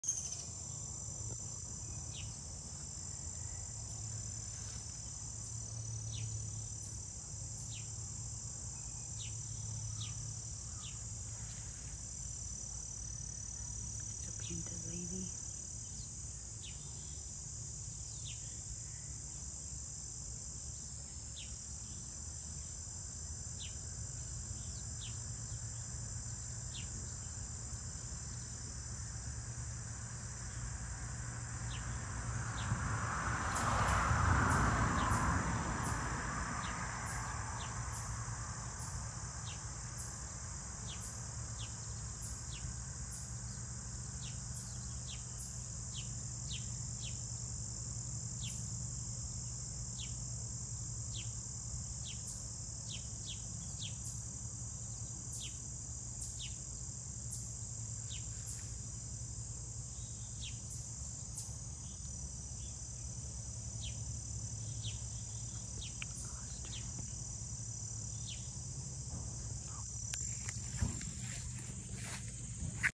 Painted lady butterfly
A beautiful painted lady butterfly she really liked showing off her colors today thank you for watching please hit the bell hit the like button and subscribe to my channel video is copyrighted thank you for watching have a great day